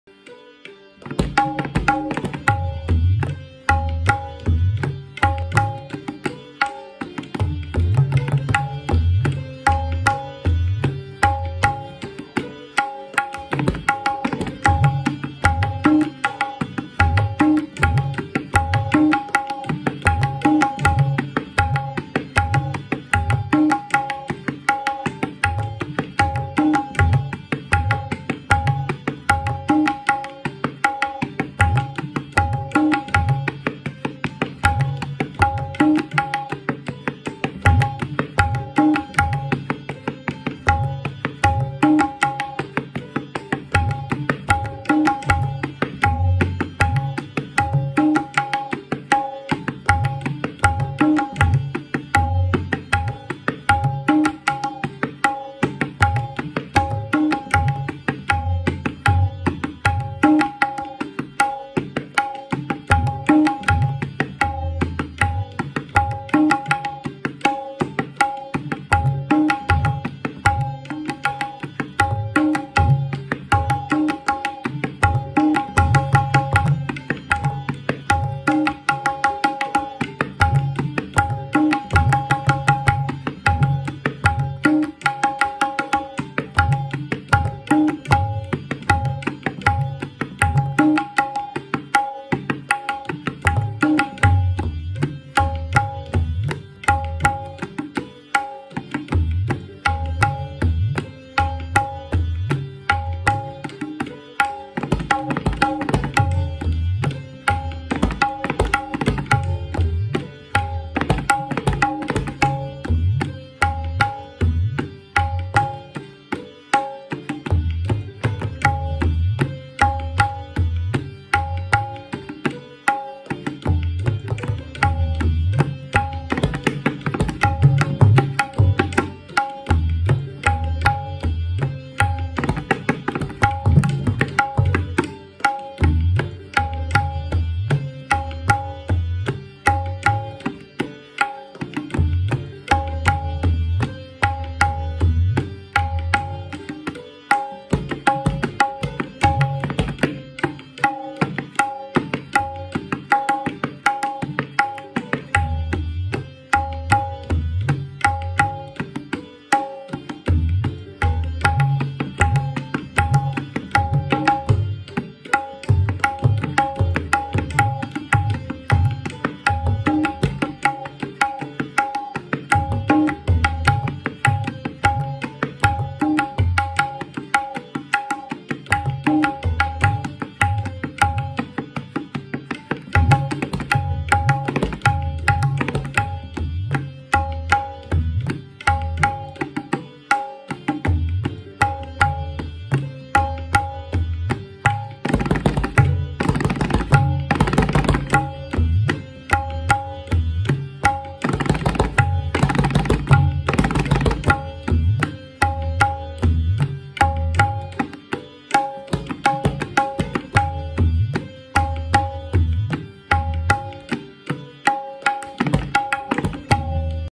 Teen taal kaida
MUSIC ROOM VIDEOS
Teentaal kaida with few variations
Practicing session